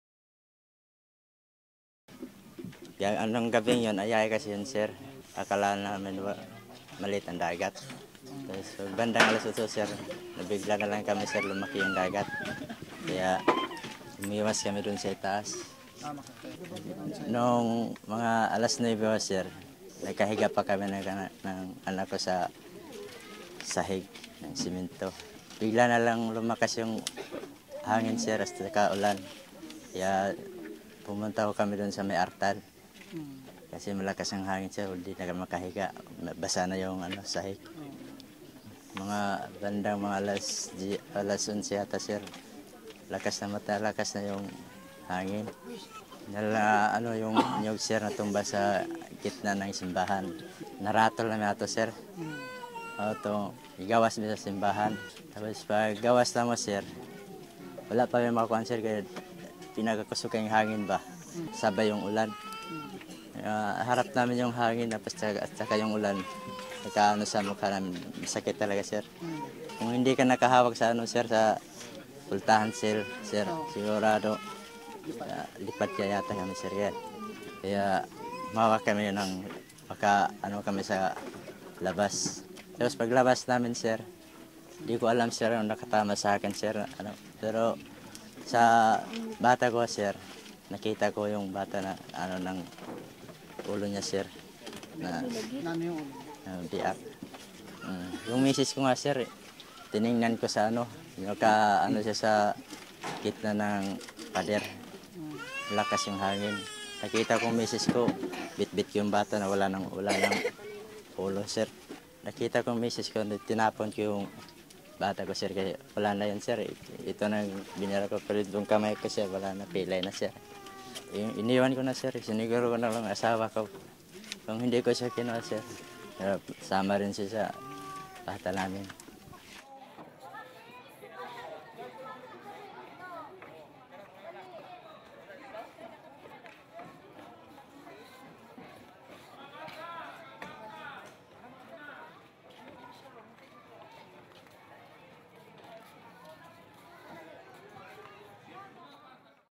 Surviving Pablo (Part I)
Richard Masamayor, a typhoon Pablo (Bopha) survivor in Banganga, Davao Oriental, narrates how the extreme typhoon devastated the town, how he lost his son, and how he and the rest of his family were able to survive.